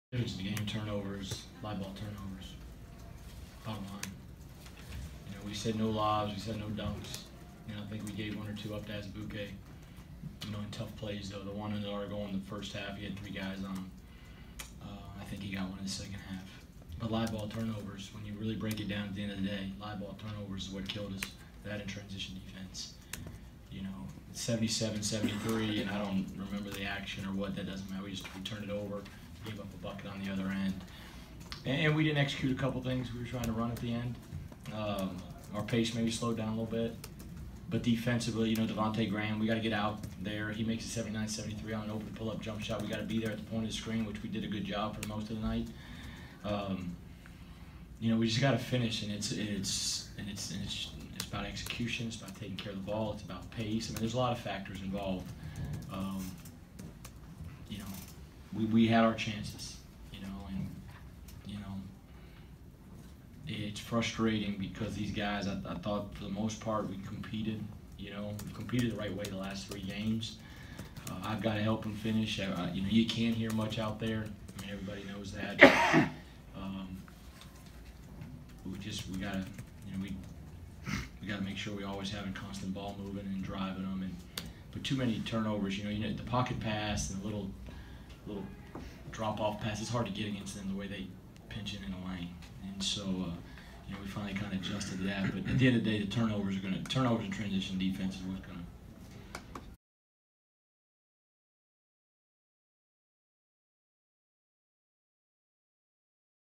Steve Prohm on Iowa State's late-game problems
Steve Prohm on Iowa State's stumble in the final minutes at Allen Fieldhouse.